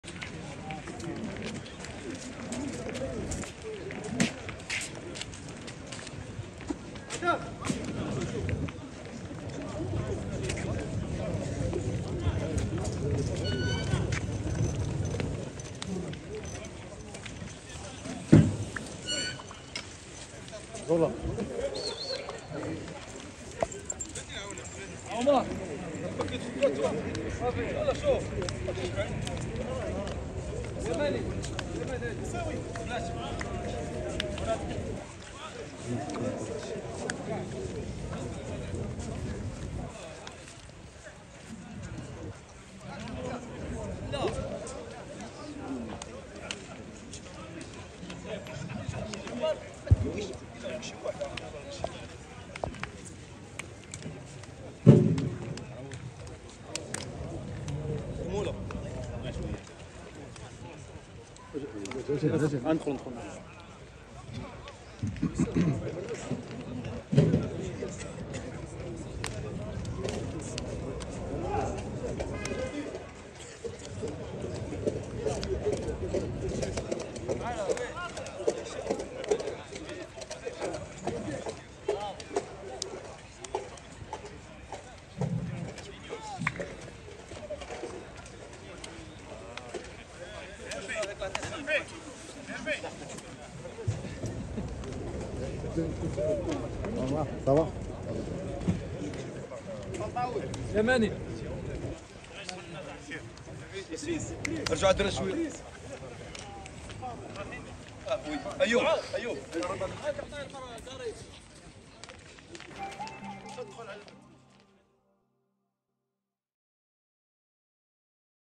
روح رياضية بين لاعبي الرجاء وشباب السوالم بعد نهاية المباراة
البطولة | حصريــــــــــــاً